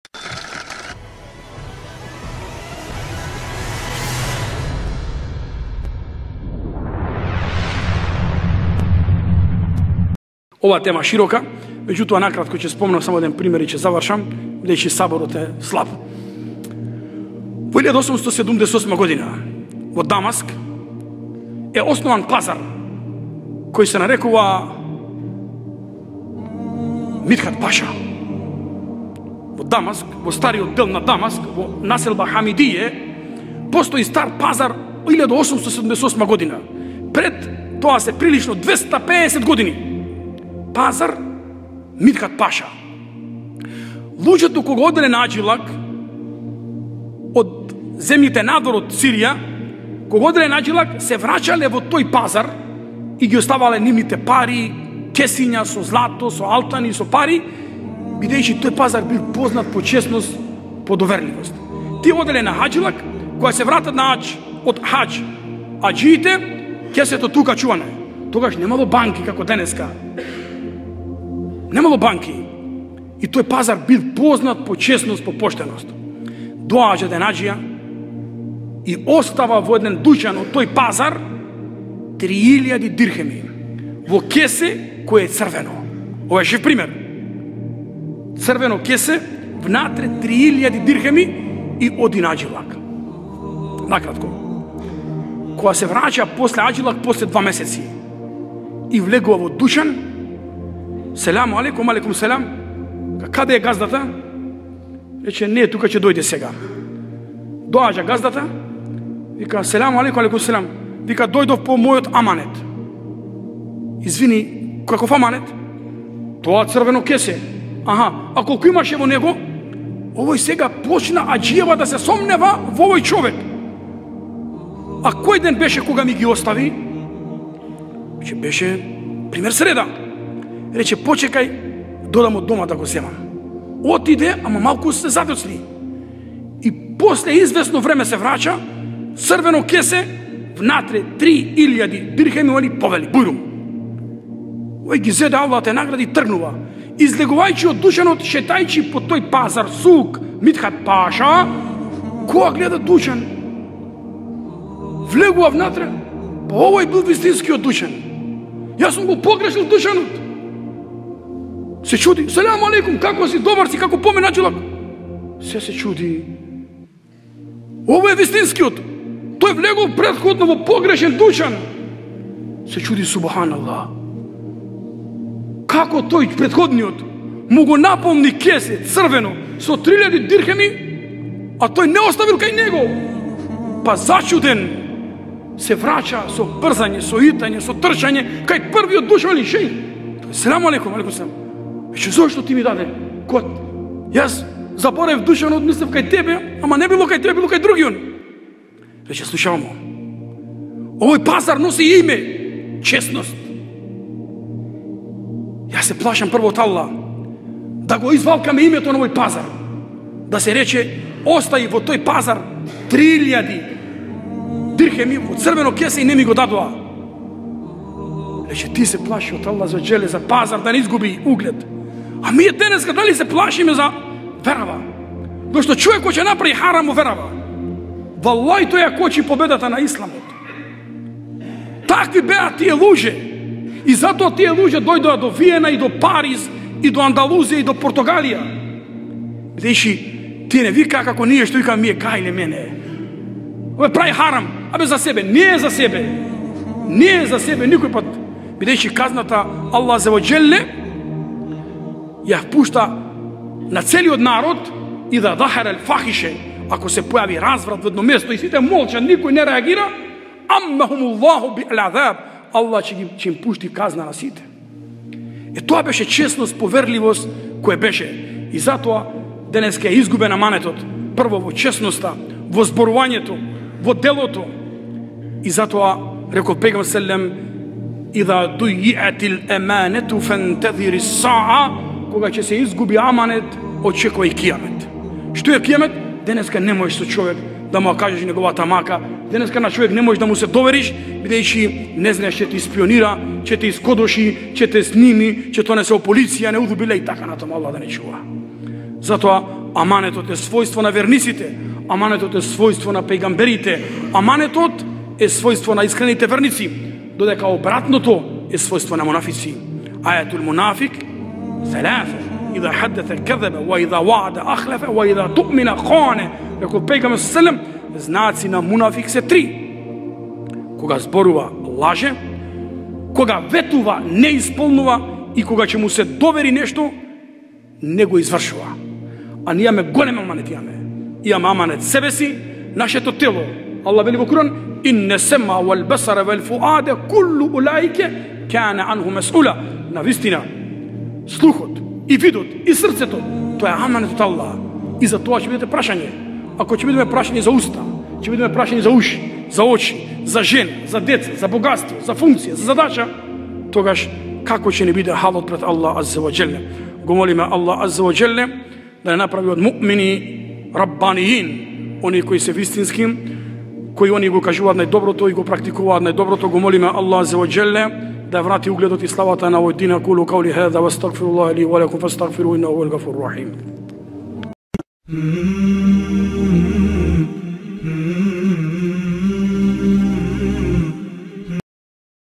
Пример од Дамаск за чесноста на поранешните муслимани..
Досега не кажано!
Кога ќе се изгуби еманетот - очекувај КИЈАМЕТ
Знаци на мунафик...
Друго...